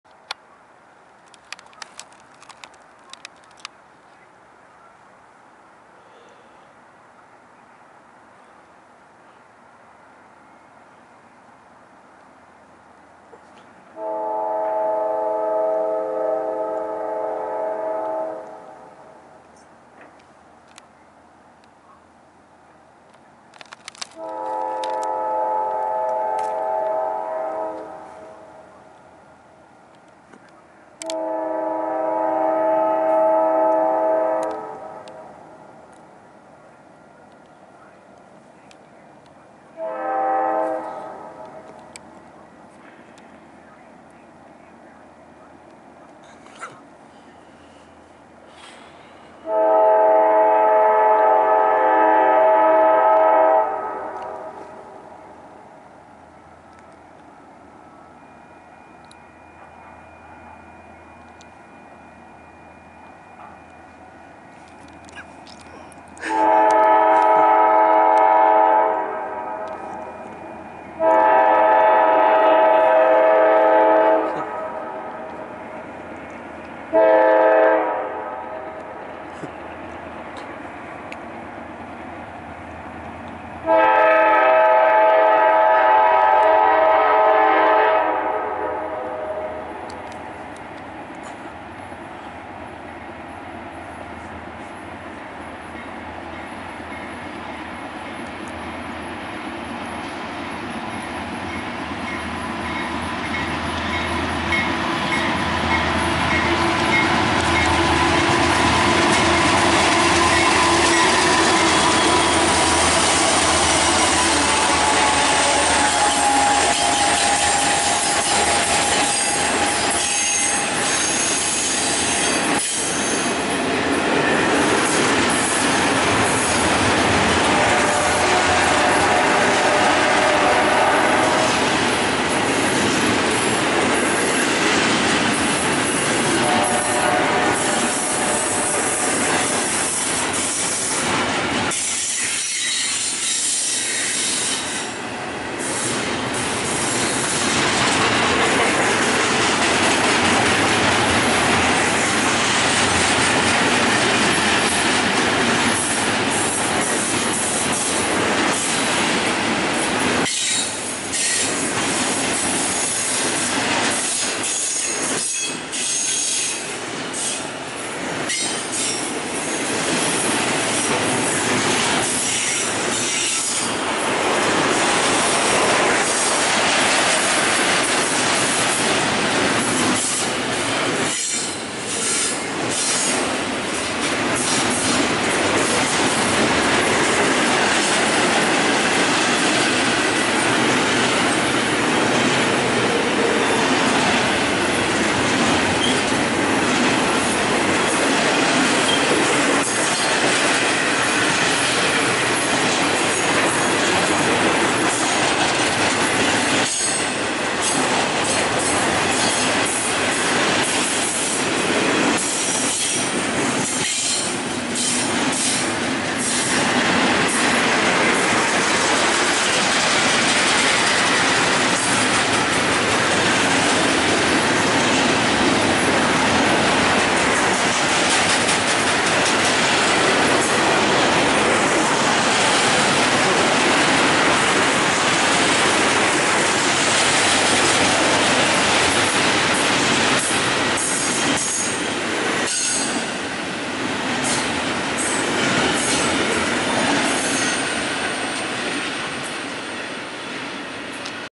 CSX 9005 leads an all CSX lash up passed Ontario
CSX 9005 (C44-9W), CSX 4836 (SD70ACE) & CSX 8527 (SD50) head Westbound passed Ontario, California on a chilly Tuesday evening. This is a rather rare lashup to see out here in SoCal. Enjoy!